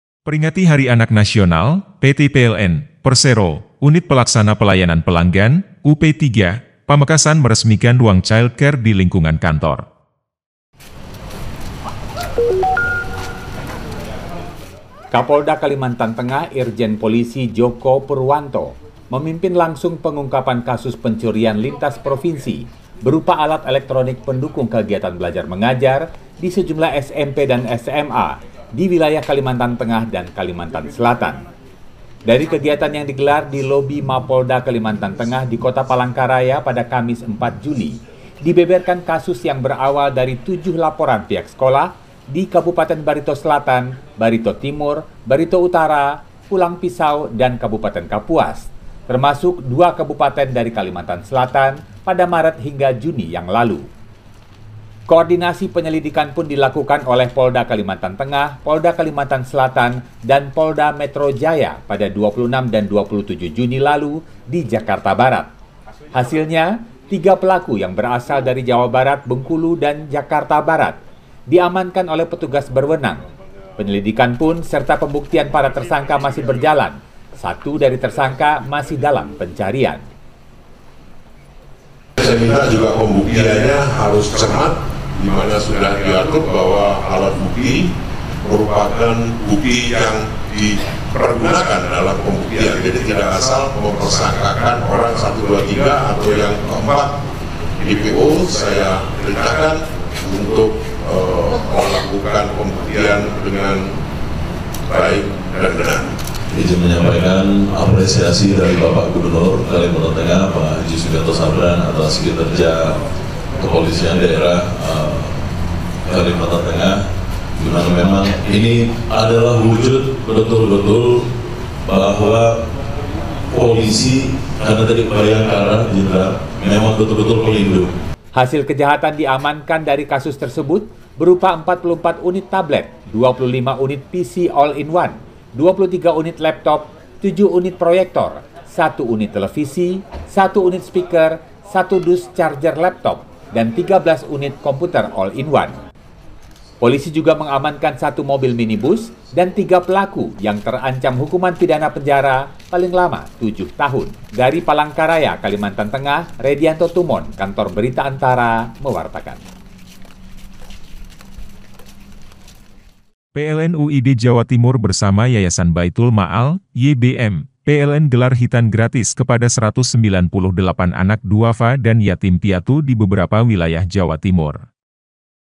Kasus Pencurian Alat Elektronik di Sekolah yang Libatkan 3 Provinsi
ANTARANEWS - Pengungkapan kasus pencurian alat elektronik pendukung kegiatan belajar mengajar di sejumlah sekolah di Kalimantan Tengah dan Kalimantan Selatan, digelar pada Kamis (4/7) oleh Kepolisian Daerah Kalimantan Tengah. Penyelidikan terhadap kasus ini melibatkan Polda Kalsel, Polda Kalteng, dan Polda Metro Jaya.